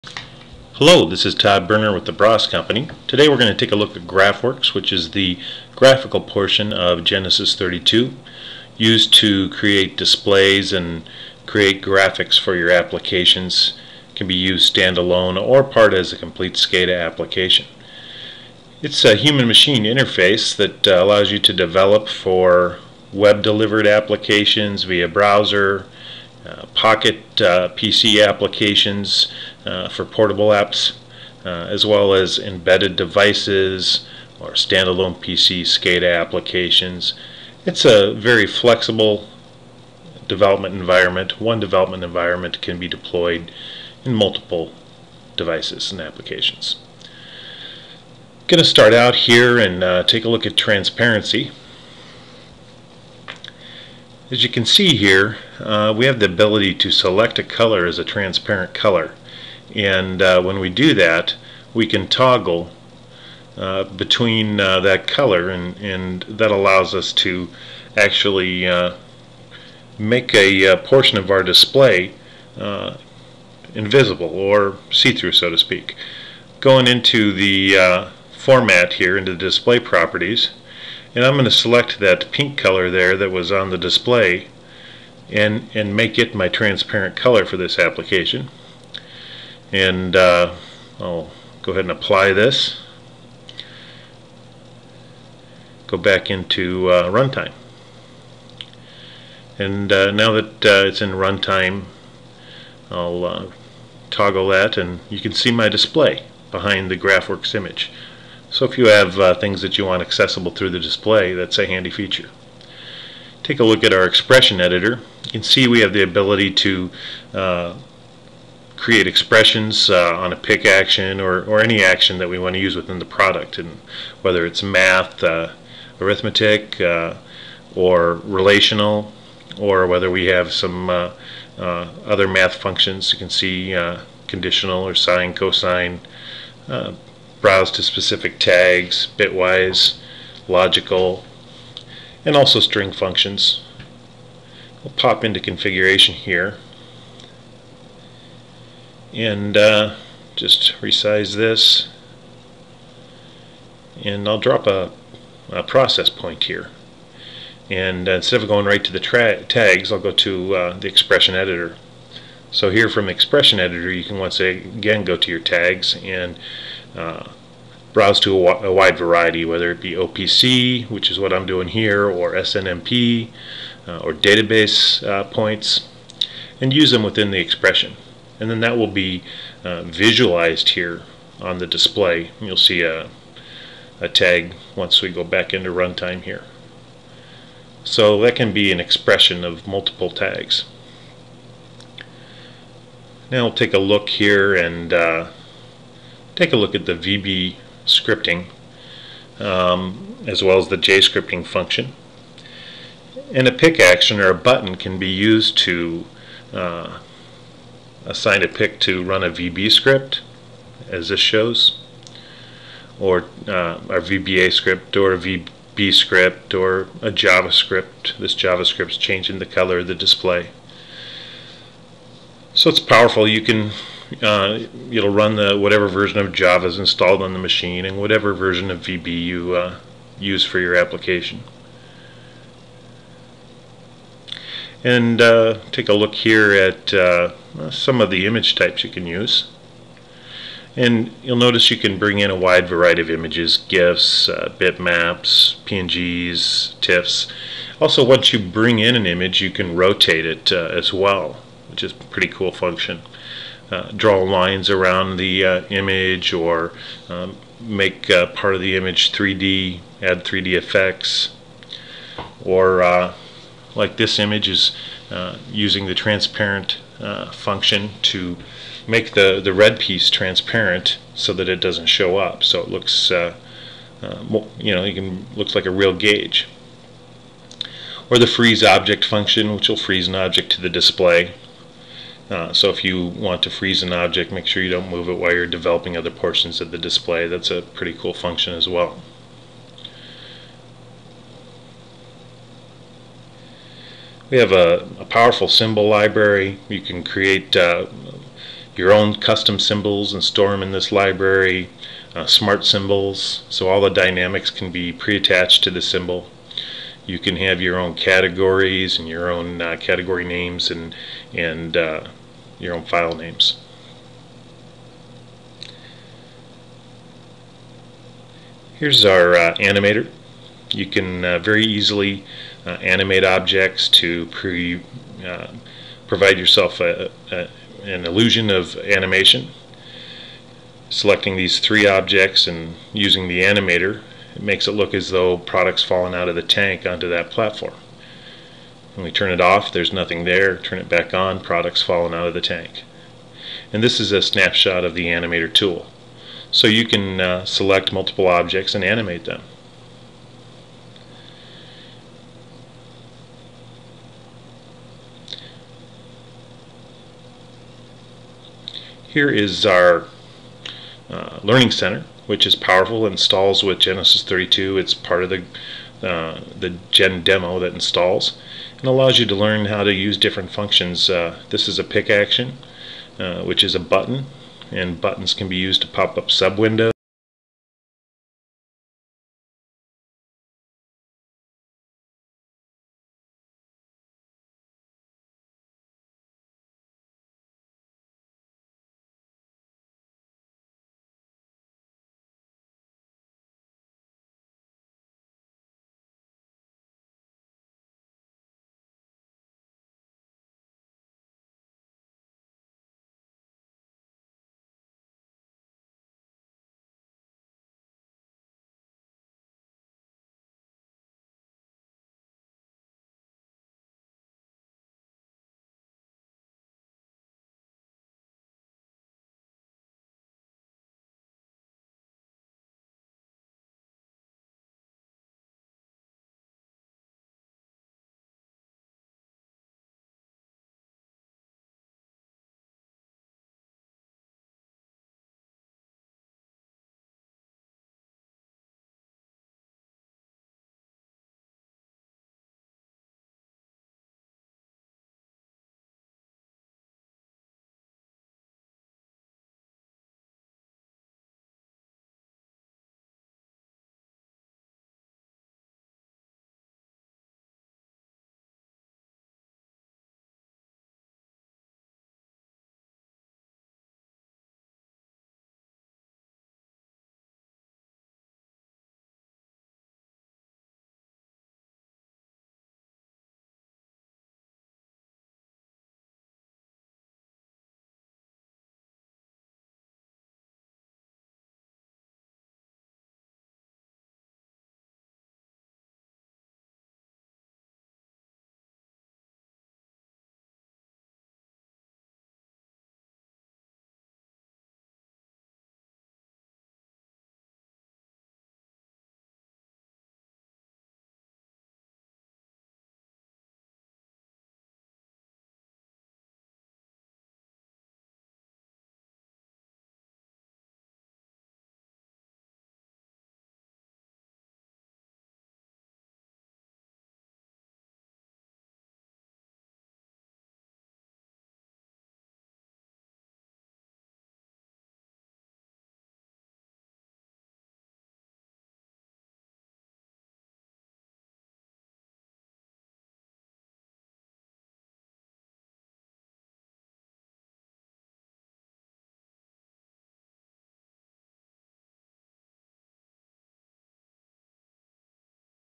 Braas Graphworx Demo
This is a overview video of the Iconics GraphWorx module from the GEN32 suite of products. Presented by Todd Berner of Braas Company.